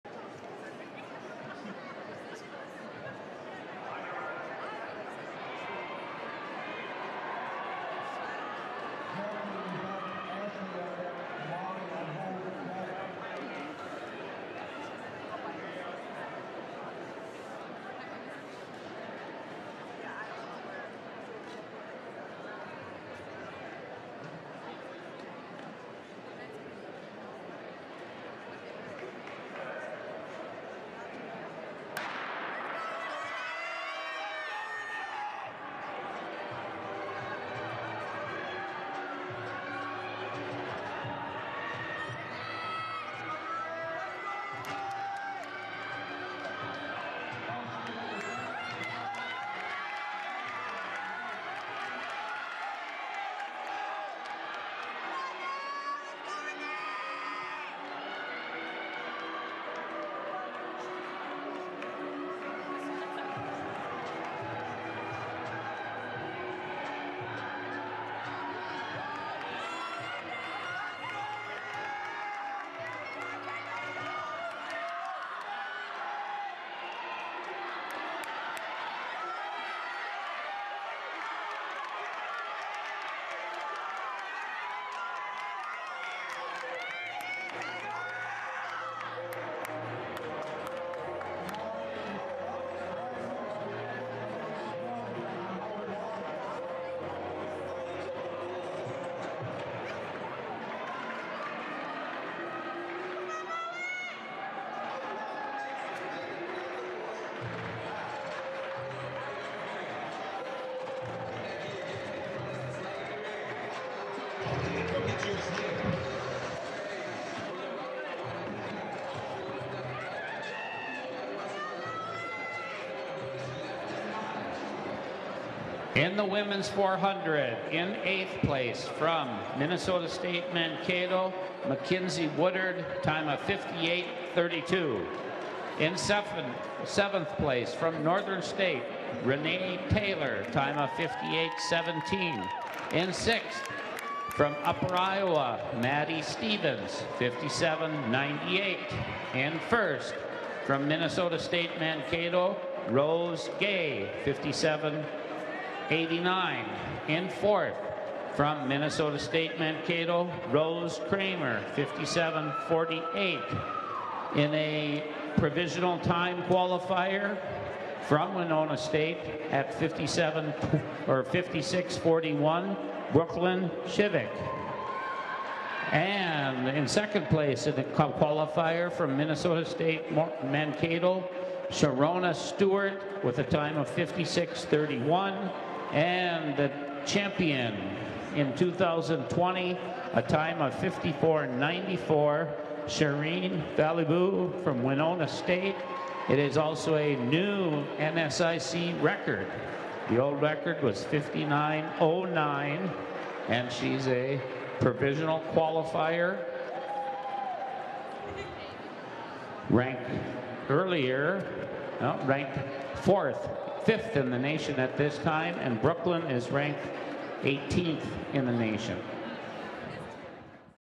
SHEREEN Vallabouy | Women 400m Dash Record | NSIC Indoor Track & Field Championship 2022
Shereen Vallabouy

MANKATO, Minn. – Minnesota State University, Mankato repeated as NSIC Indoor Track & Field Championship as the Mavericks amassed and NSIC record 243 points to win by 128 point over the University of Mary. This marks the fifth indoor women’s championship for the Mavericks. UMary was second with 115 points while Augustana was third with 106.33 points.
MSU’s Makayla Jackson, who set the NSIC record in the prelims of the 60m dash with a time if 7.36 on Friday, broke it again in the finals as she ran a 7.32. Teammate Denisha Cartwright also improved her NSIC record from Friday’s prelims in the 200m dash as she ran 24.22 in the finals. Both earned NSIC High Point Performer honors as they each scored 28 points at the championship. Eleven records were broken on the women’s side at the championships this year.
In the 400m dash, Winona State’s Shereen Vallabouy ran 54.94 to break the 2010 record held by Bemijdi State’s Kristi Buerkle (55.09). In the triple jump, University of Mary’s D’Andra Morris set a record with a leap of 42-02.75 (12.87m) to surpass the 2015 mark of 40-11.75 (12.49m) jumped by Danylle Kurywchak (U-Mary).
Five of the records fell on Friday, with four being set by MSU, and two of those by Makayla Jackson and Denisha Cartwright.  Jackson won the long jump again with a provisional leap of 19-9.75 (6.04m). She broke her NSIC record of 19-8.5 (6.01m) from 2020. Jackson also set the NSIC record in the prelims of the 60m dash as she ran a NCAA provisional qualifying time of 7.36, eclipsing her 2020 time of 7.43. Cartwright set a new mark in the 60m hurdle prelims as she ran 8.23 to break the 2018 record of 8.29 ran by UMD’s Danielle Kohlwey. She later ran 24.36 in the 200m prelims to break her own record of 24.47 ran in 2021 and by MSU’s Ja’Cey Simmons in 2020. The final record to fall on Friday happened when Arika Robinson of Concordia-St. Paul won the women’s high jump as she cleared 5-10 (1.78m) breaking the 2015 mark of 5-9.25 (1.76m) set by Augustana’s Bree Woelber.
The top three finishers in each event and the top two relay teams are named to the NSIC Indoor Track & Field All-Conference team. NSIC Indoor Track & Field Athletes and Coaches of the Year will be announced next week at NorthernSun.org.
The NCAA National Championships will take place on March11-12 at the Robert W. Plaster Center in Pittsburg, Kansas. The field of student-athletes participating will be announced on Tuesday, March 1 at approximately 5 p.m.